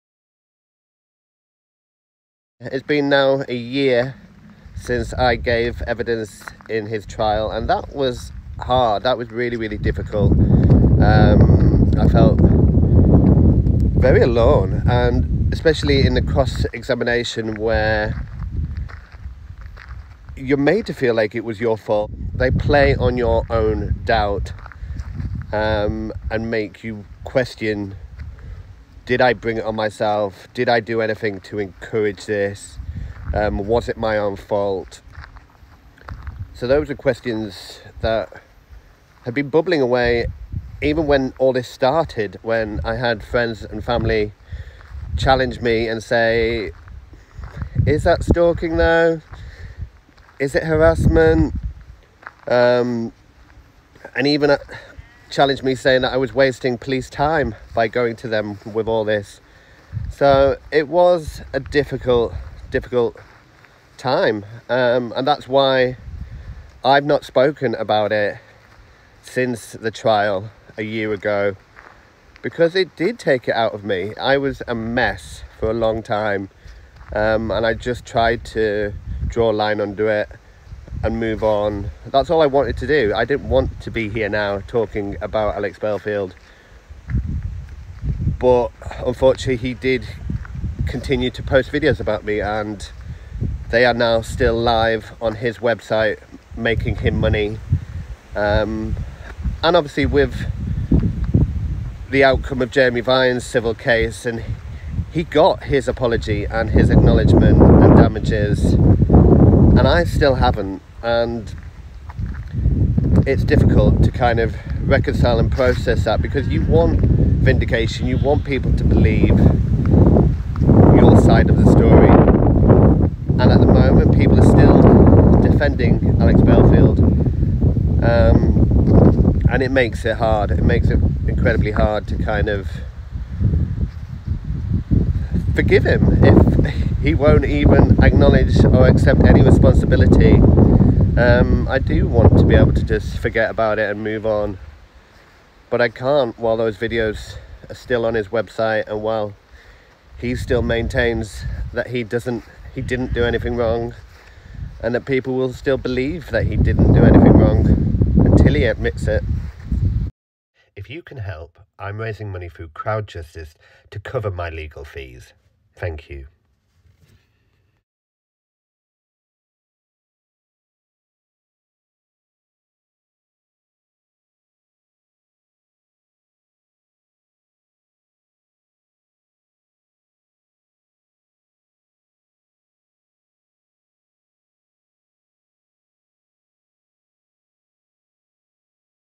Alex Belfield Convicted Stalker: One year since I gave evidence
It is now one year since I gave evidence against Alex Belfield in his trial for stalking. I haven’t spoken out about this until now.

These are two news articles that were published at the time when I gave evidence.

Although being convicted for stalking and settling a defamation claim with Jeremy Vine, Belfield still refuses to acknowledge the damage he causes to me.

Please help me to raise legal fees to claim compensation from ex BBC presenter Alex Belfield  @AlexBelfieldTheVoiceOfReason  who harassed and stalked me, and then threatened “he’d be back”.

I am raising money through Crowd Justice. All the money raised will go directly to Samuel Solicitors.

I am Philip Dehany and I need your help. I was a theatre blogger and am now a volunteer for the Terrence Higgins Trust.

I am one of the several victims who were harassed by former BBC presenter Alex Belfield, which led to him being jailed for five and a half years. After his trial concluded, but before he was sentenced, Belfield publicly revealed information about my HIV status. I am now claiming compensation from him in the civil courts, under the Protection from Harassment Act 1997.

Belfield’s awful campaign of harassment against me, (much of which was directed at my sexuality) continued even during and after his trial. He constantly published harassing videos about me and others, and made a lot of money from YouTube advertising revenue and by asking for donations from his followers.

The impact upon me has been huge - my reputation has been seriously damaged and I lost my passion, confidence and ultimately my income as a theatre blogger. I am now seeking therapy.

I have already spent over £4,000 in legal fees and have to pay £1,500 more for the work which has been done to get me to this stage.

Unless Belfield agrees to settle with me, which seems very unlikely, I will have no choice but to issue court proceedings against him and my costs to take the matter all the way to trial could reach £100,000.

If you are able to help, donate or share my story, thank-you.

ALEX BELFIELD: The Truth, The Whole Truth, and Nothing But The Truth

I will be continue to update this article with evidence until this case is resolved.

Further reading

These are the sentencing remarks from the case ‘R v Belfield’ detailing the full case:

This article was published in The Guardian including an interview I did with Helen Pidd.

This article was published in the Guardian after Belfield was sentenced.

This article was published in the Guardian after Belfield was found guilty.